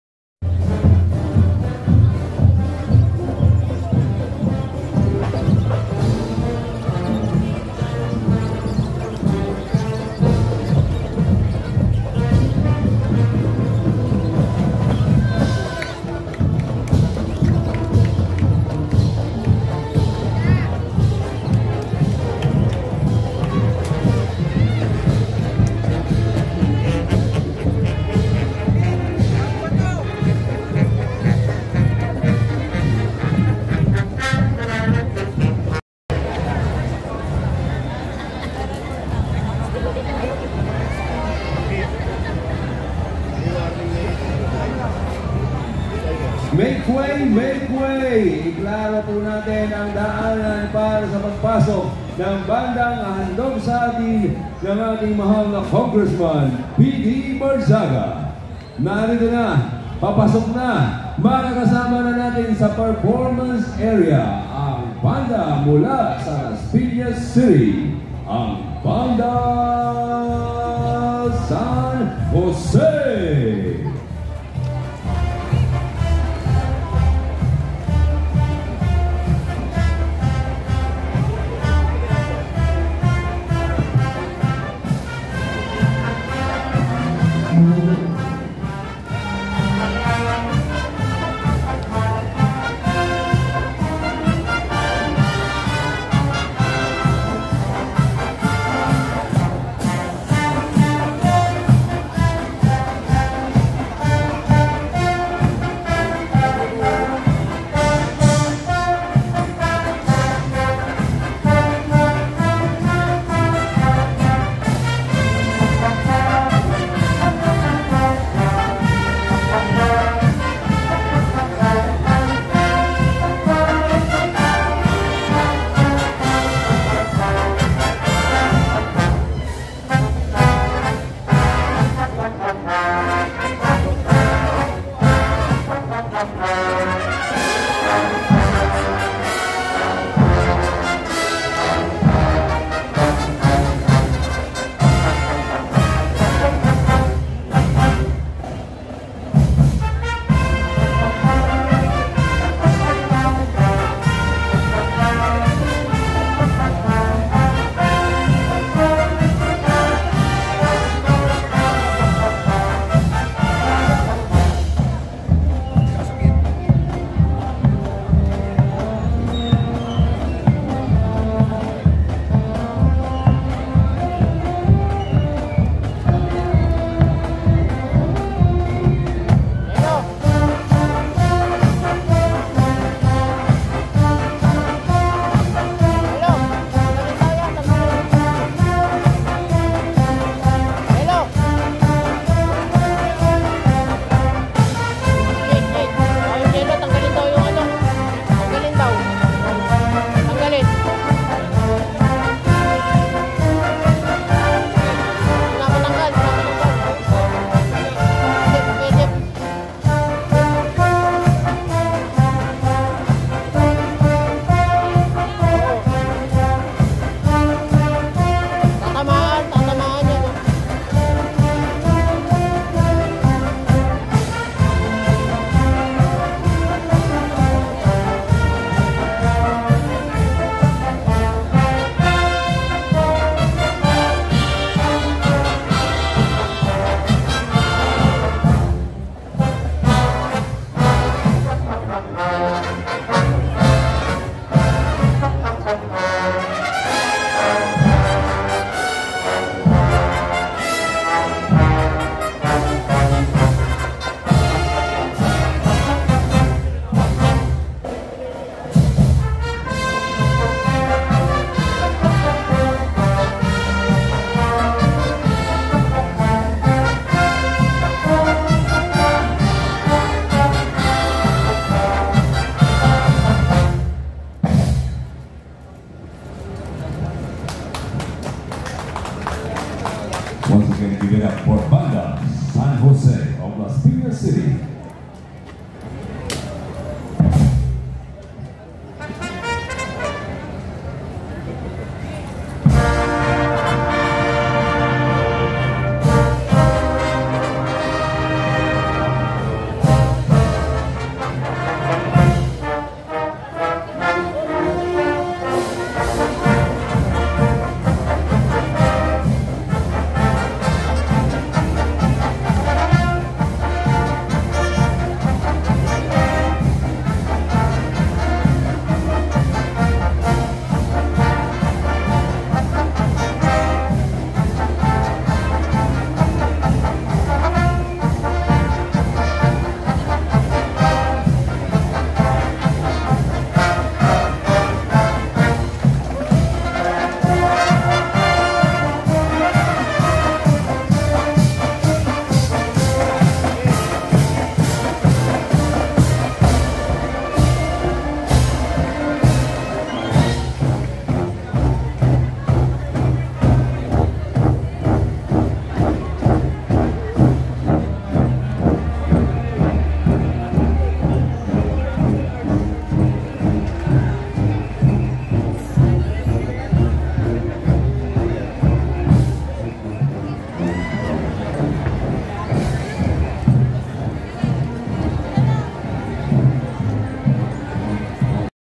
Banda San Jose- Las Piñas City Immaculate conception town fiesta 2022 DASMARINAS city cavite
Banda San Jose- Las Piñas City
Immaculate conception town fiesta 2022 DASMARINAS city cavite
Grand parade Band
Cbbd Cbdband
Musikong Pilipino